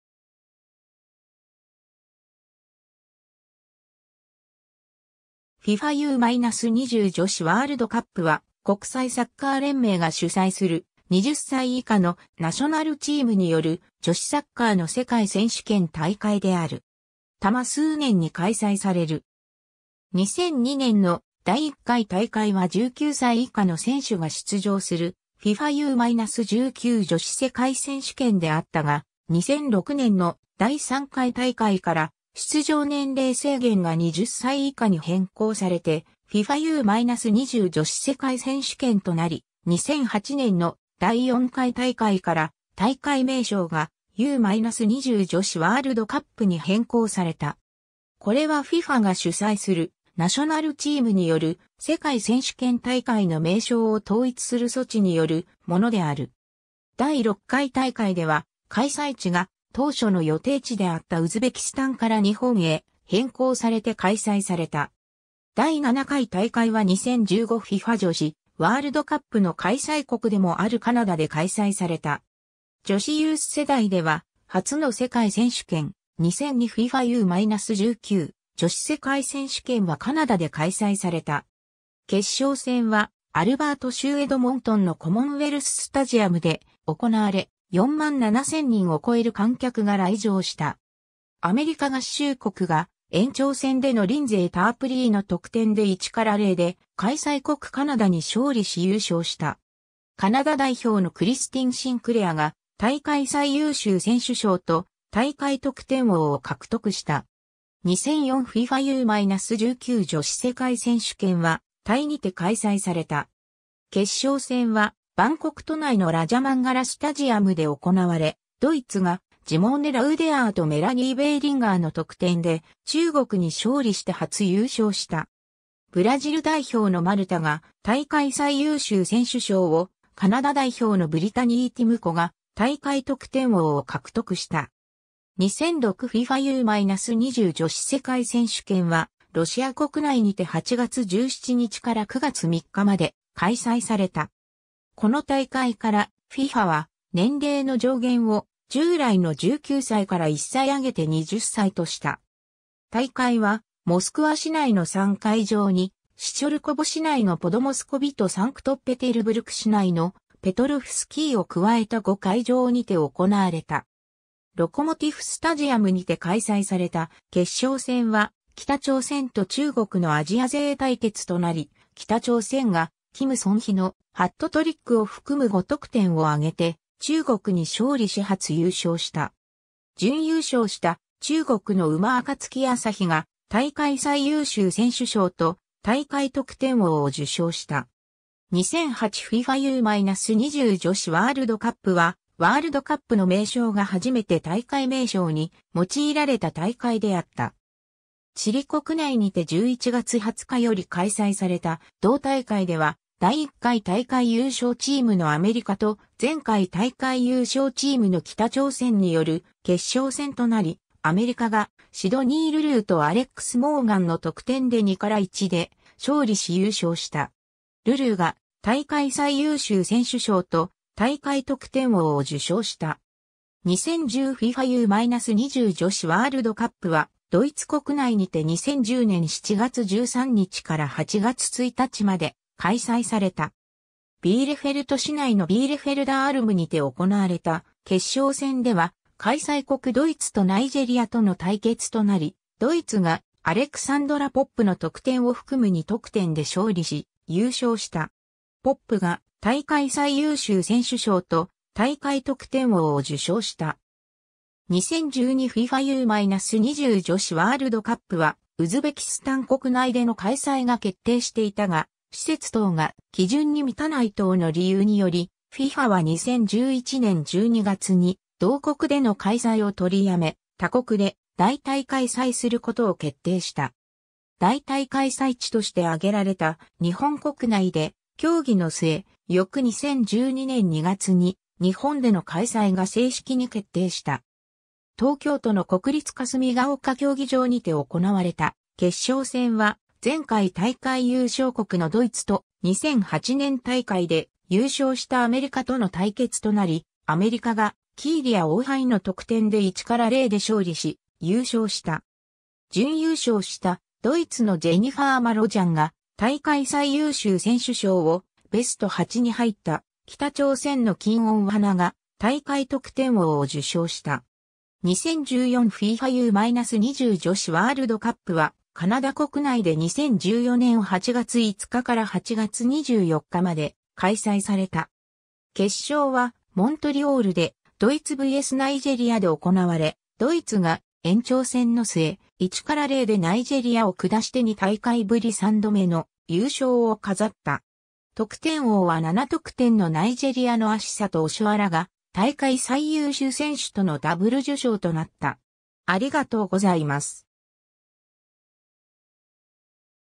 FIFA U-20女子ワールドカップ
FIFA U-20女子ワールドカップ（英: FIFA U-20 Women's World Cup）は、国際サッカー連盟（FIFA）が主催する、20歳以下のナショナルチームによる女子サッカーの世界選手権大会である。
偶数年に開催される。
2002年の第1回大会は19歳以下の選手が出場するFIFA U-19女子世界選手権であったが、2006年の第3回大会から出場年齢制限が20歳以下に変更されてFIFA U-20女子世界選手権となり、2008年の第4回大会から大会名称がU-20女子ワールドカップに変更された。
これはFIFAが主催するナショナルチームによる世界選手権大会の名称を統一する措置によるものである。
 第6回大会では開催地が当初の予定地であったウズベキスタンから日本へ変更されて開催された（後述）。
第7回大会は2015 FIFA女子ワールドカップの開催国でもあるカナダで開催された。
 女子ユース世代では初の世界選手権、2002 FIFA U-19女子世界選手権はカナダで開催された。
決勝戦はアルバート州エドモントンのコモンウェルス・スタジアムで行われ、47,000人を超える観客が来場した。
アメリカ合衆国が延長戦でのリンゼイ・タープリーの得点で1-0で開催国カナダに勝利し優勝した。
カナダ代表のクリスティン・シンクレアが大会最優秀選手賞（ゴールデンボール）と大会得点王（ゴールデンシューズ、10得点）を獲得した。
 2004 FIFA U-19女子世界選手権はタイにて開催された。
決勝戦はバンコク都内のラジャマンガラ・スタジアムで行われ、ドイツがジモーネ・ラウデアーとメラニー・ベーリンガーの得点で中国に勝利して初優勝した。
ブラジル代表のマルタが大会最優秀選手賞を、カナダ代表のブリタニー・ティムコが大会得点王を獲得した。
 2006 FIFA U-20女子世界選手権は、ロシア国内にて8月17日から9月3日まで開催された。
この大会から、FIFAは年齢の上限を従来の19歳から1歳上げて20歳とした。
大会はモスクワ市内の3会場（ディナモ、ロコモティフ、トルペド）にシチョルコヴォ市内のポドモスコヴィとサンクトペテルブルク市内のペトロフスキーを加えた5会場にて行われた。
 ロコモティフ・スタジアムにて開催された決勝戦は北朝鮮と中国のアジア勢対決となり、 北朝鮮がキム・ソンヒのハットトリックを含む5得点を挙げて中国に勝利し初優勝した。
準優勝した中国の馬暁旭が大会最優秀選手賞と大会得点王（6得点）を受賞した。
 2008 FIFA U-20女子ワールドカップは「ワールドカップ」の名称が初めて大会名称に用いられた 大会であった。
チリ国内にて11月20日より開催された同大会では、第1回大会優勝チームのアメリカと、前回大会優勝チームの北朝鮮による決勝戦となり、アメリカがシドニー・ルルーとアレックス・モーガンの得点で2-1で勝利し優勝した。
ルルーが大会最優秀選手賞と大会得点王を受賞した。
 2010 FIFA U-20女子ワールドカップは、ドイツ国内にて2010年7月13日から8月1日まで開催された。
ビーレフェルト市内のビーレフェルダー・アルム（Bielefelder Alm）にて行われた決勝戦では、開催国ドイツとナイジェリアとの対決となり、ドイツがアレクサンドラ・ポップの得点を含む2得点で勝利し優勝した。
ポップが大会最優秀選手賞と大会得点王を受賞した。
 2012 FIFA U-20女子ワールドカップはウズベキスタン国内での開催が決定していたが、施設等が規準に満たない等の理由により、FIFAは2011年12月に同国での開催を取りやめ、他国で代替開催する事を決定した。
代替開催地として挙げられた日本 国内で協議の末、翌2012年2月に日本での開催が正式に決定した。
 東京都の国立霞ヶ丘競技場にて行われた決勝戦は、前回大会優勝国のドイツと、2008年大会で優勝したアメリカとの対決となり、アメリカがキーリア・オーハイの得点で1-0で勝利し優勝した。
準優勝したドイツのジェニファー・マロジャンが大会最優秀選手賞を、ベスト8に入った北朝鮮の金恩花が大会得点王を受賞した。
 2014 FIFA U-20女子ワールドカップはカナダ国内で2014年8月5日から8月24日ま...